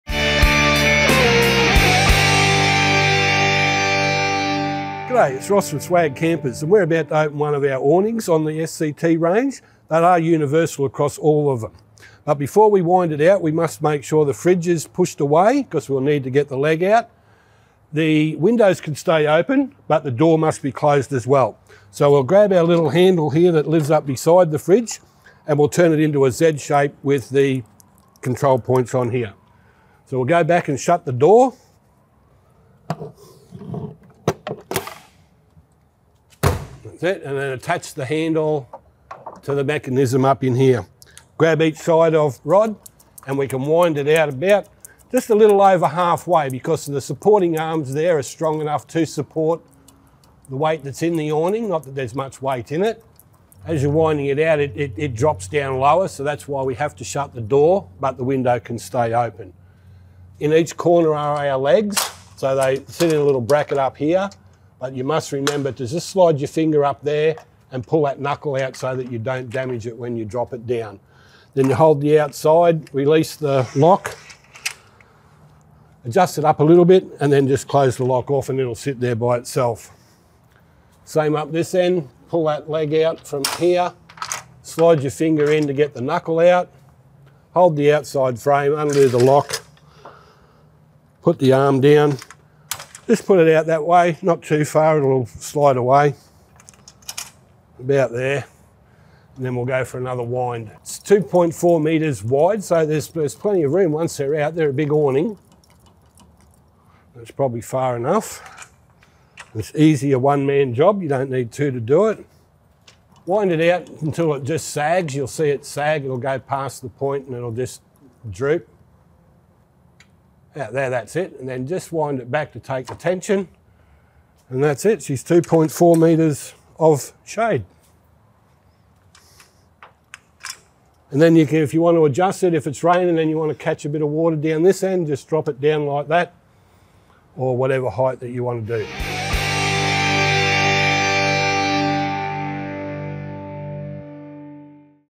Swag Instructional Series: AWNINGS
Welcome to SWAG's Instructional Series! 🎥

Ready to master the art of using awnings on your Swag Off-Road Camper? In this video, we've got you covered with step-by-step instructions and expert tips to make the most of your outdoor space.